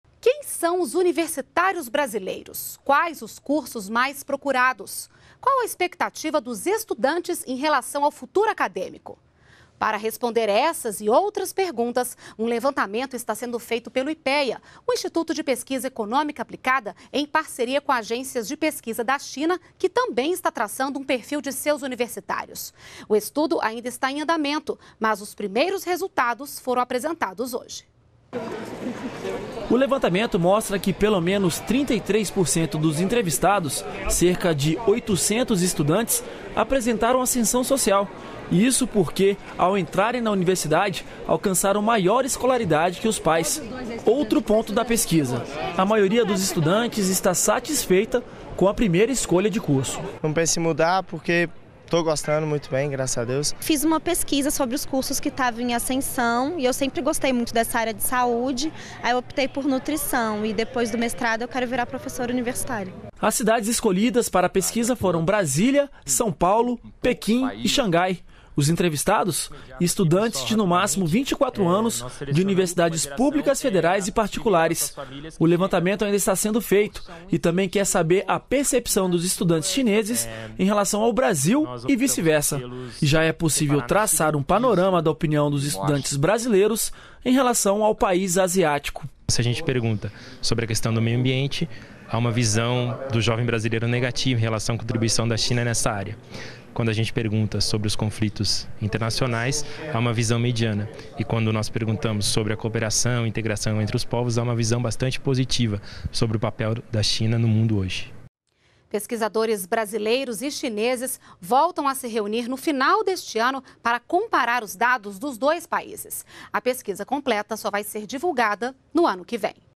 China e Brasil analisam perfil de estudantes universitários de grandes cidades
NBR NOTÍCIAS - 06.11.12: O Instituto de Pesquisa Econômica Aplicada (Ipea), em parceria com os institutos de pesquisas chineses, vai analisar o perfil de estudantes universitários de até 24 anos de instituições públicas e particulares de quatro cidades: São Paulo, Brasília, Pequim e Xangai. As informações dos países serão comparadas ainda em 2012. Os primeiros números da pesquisa revelam que 33% dos entrevistados apresentaram ascensão social e satisfação com a escolha do curso. O resultado final do levantamento será divulgado em 2013.